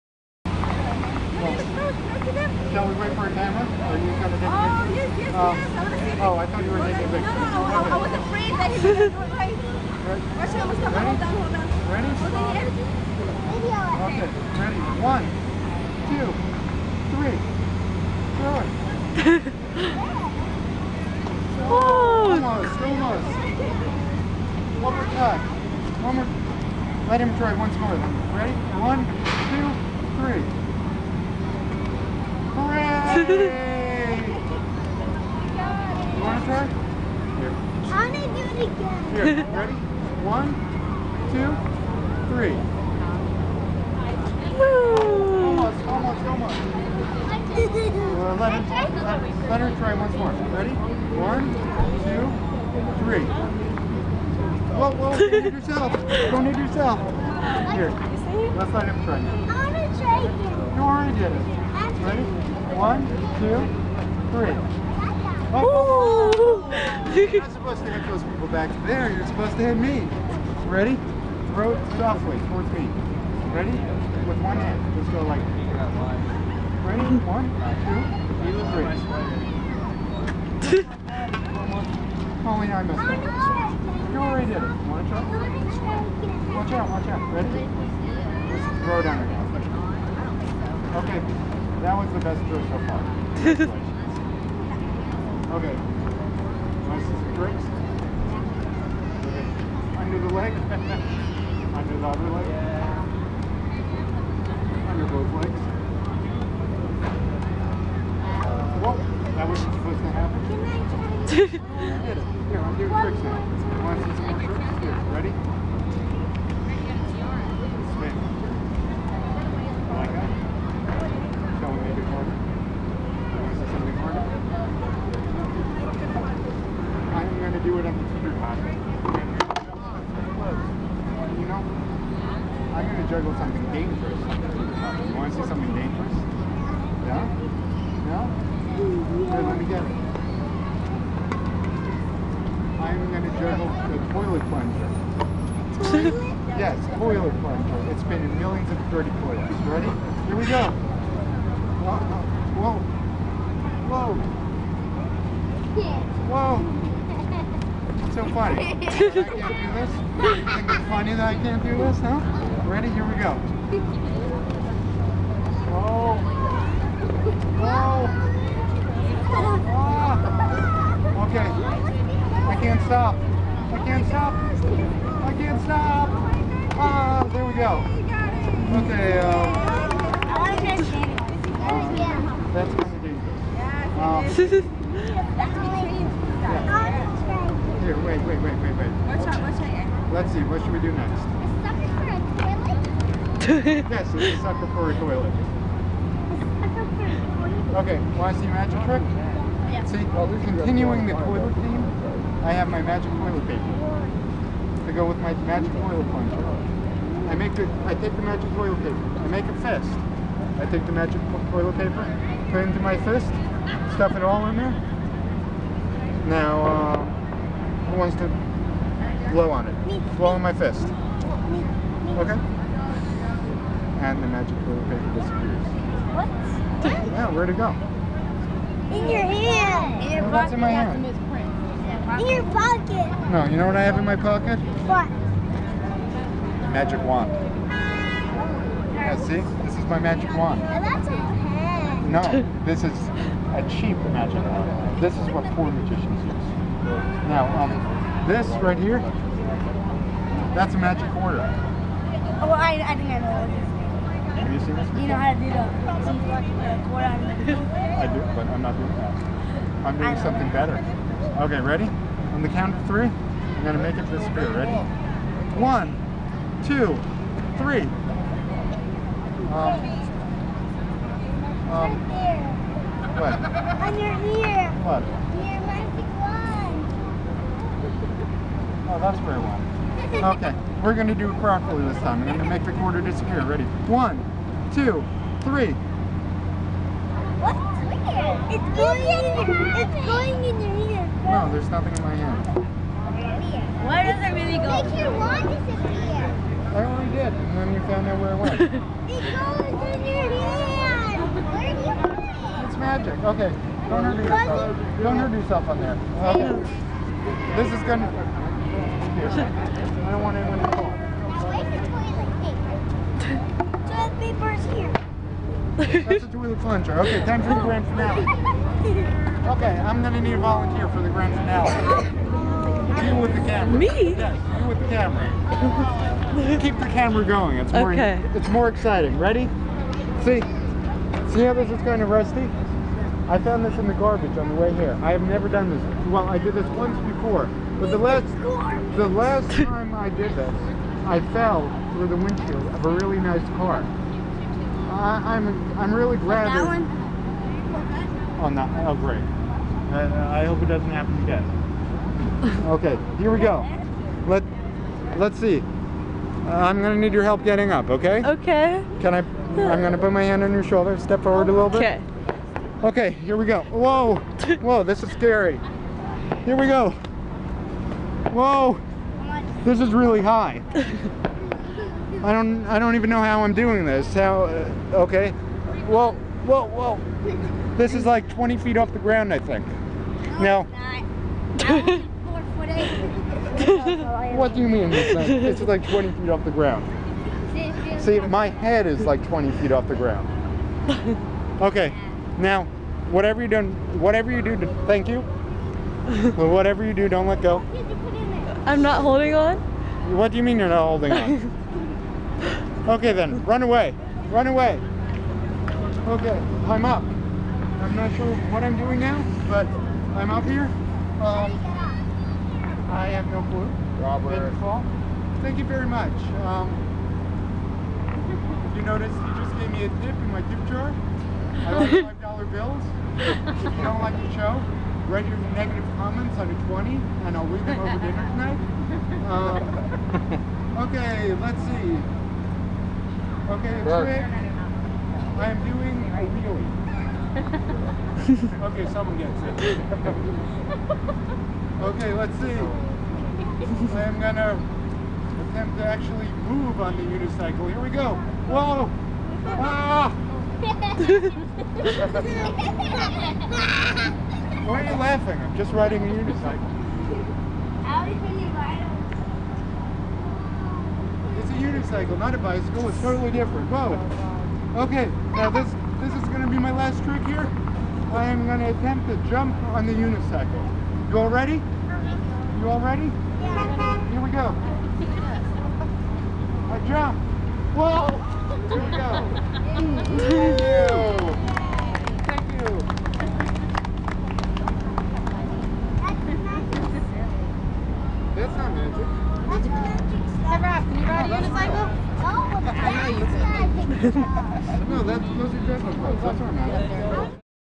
Marketable Skills
This was one of my first successful busking attempts at Santa's Winter Wonderland Village in Waterford Lakes. This show format of juggling/magic/unicycle seemed to work well in the carnival environment for street performing!